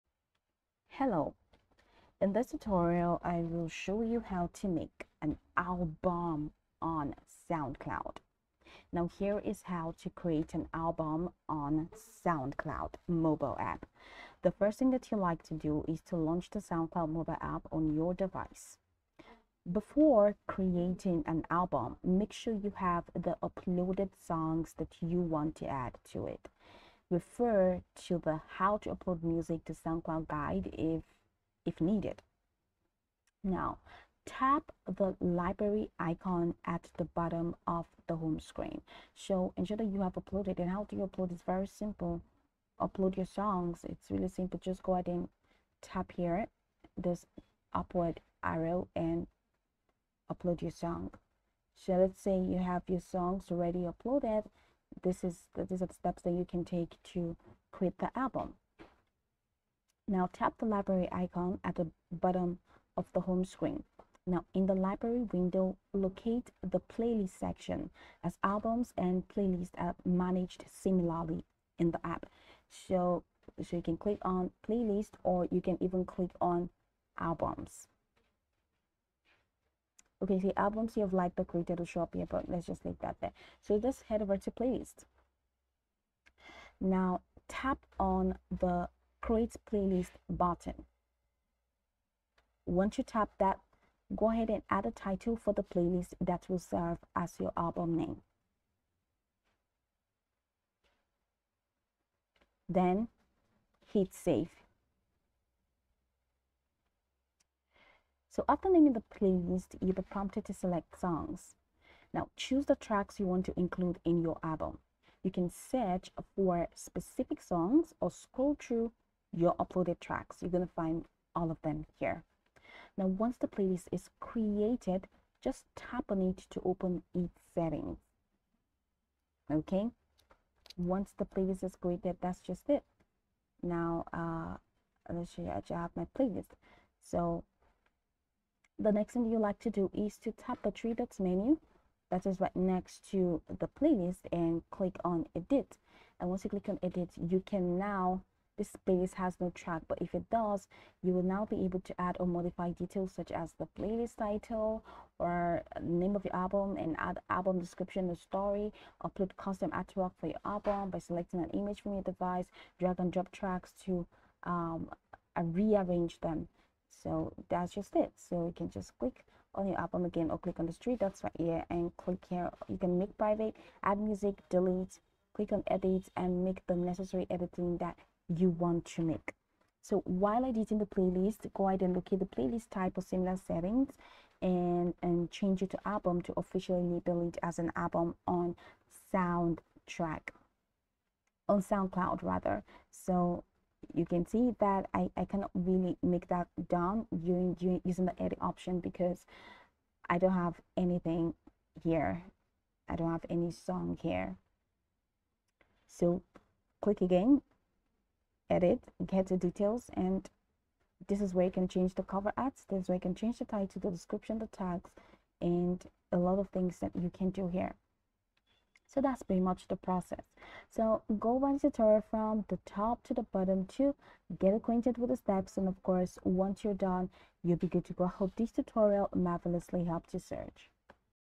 How To Make an Album on Soundcloud (Quick Tutorial)
In this video I will show you How To Make an Album on Soundcloud. It would be good if you watch the video until the end so that you don't miss important steps.